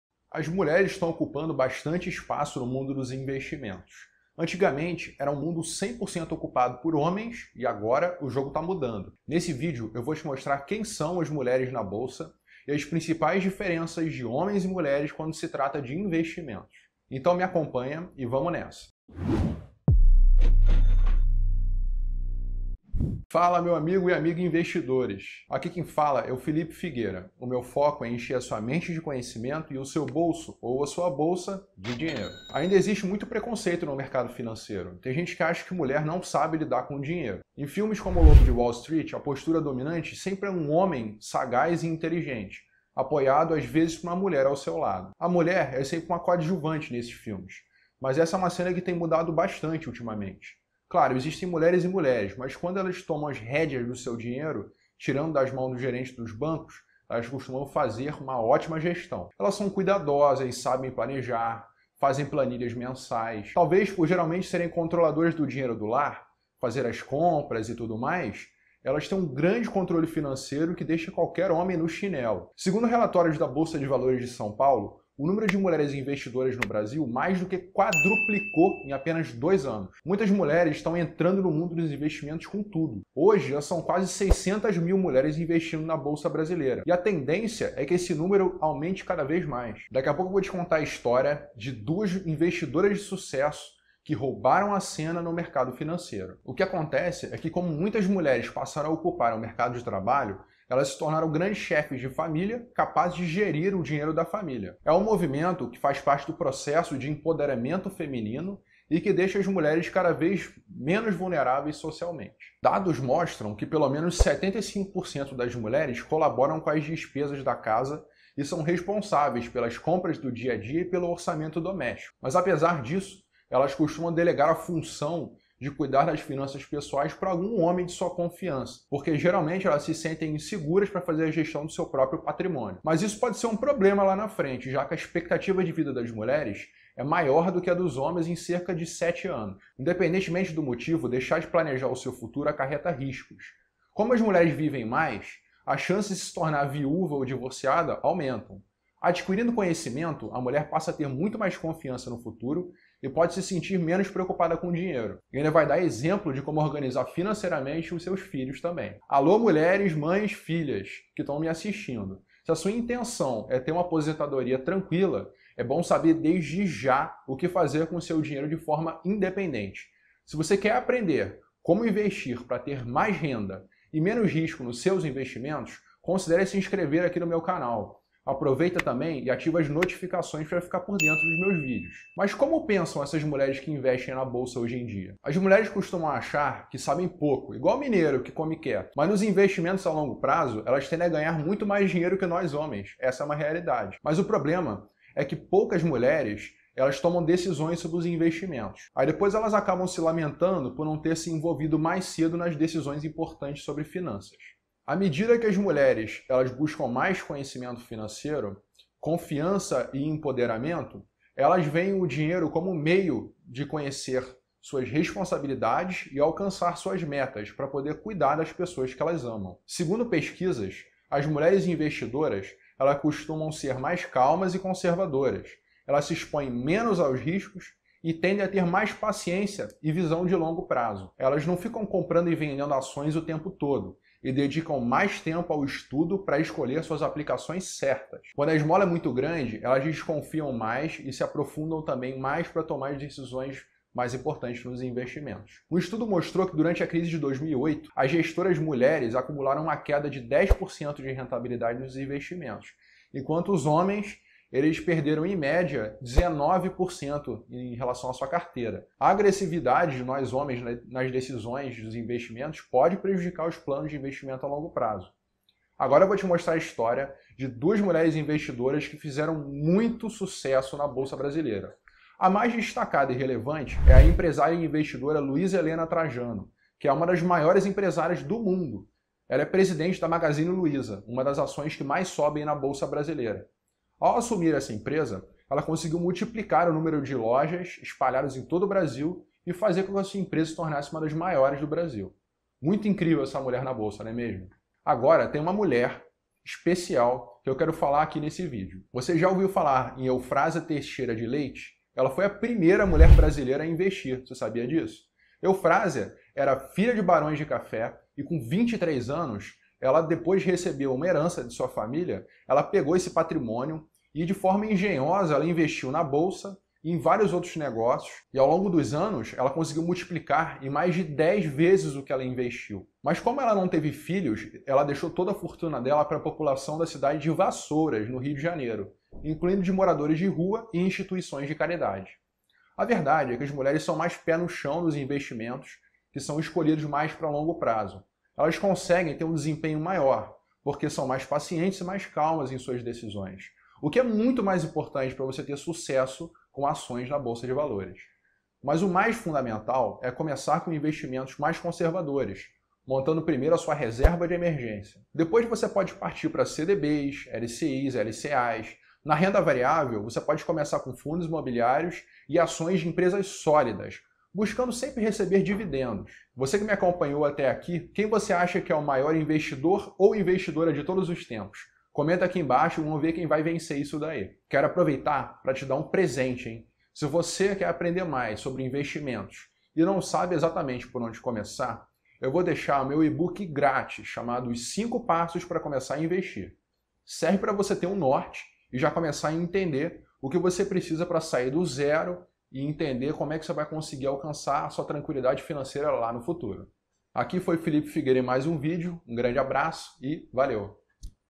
Veja quem são as Mulheres Investidoras que estão arrasando na Bolsa!
Homens ou Mulheres: quem investe Melhor na Bolsa?
Gabriela Pugliesi Trader tem quebrado os paradigmas comprando ações na Bolsa.
Mas mulher geralmente é mais paciente nas posições de ações para longo prazo. Homens costumam ser mais agressivos e podem comprometer tudo..

O empoderamento feminino tem feito muitas mulheres participarem de suas decisões financeiras ativamente.
E o mundo tem dado diversos exemplos de mulheres investidoras bem sucedidas e empoderadas. São como exemplos a dona da Magazine Luiza, Luiza Heleno Trajano.
E pra você: quem é a(o) melhor investidor(a)?